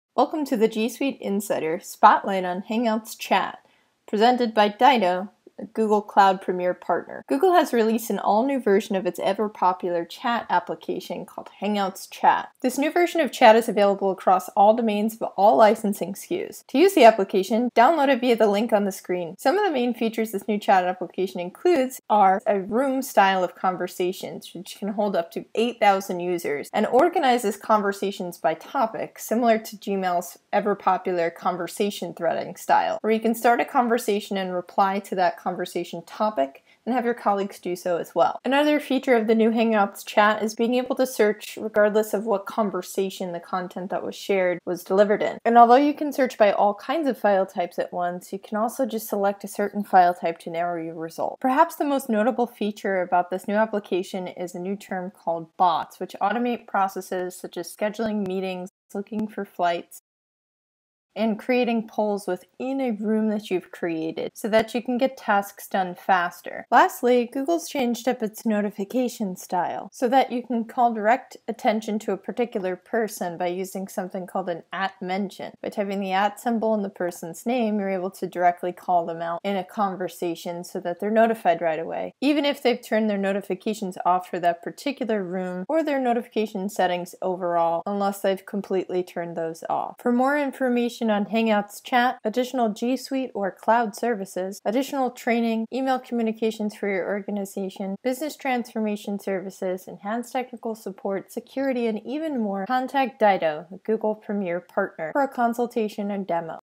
Google Hangouts Chat overview demo
Get a quick overview of the enhanced, newly released version of Hangouts Chat for G Suite users.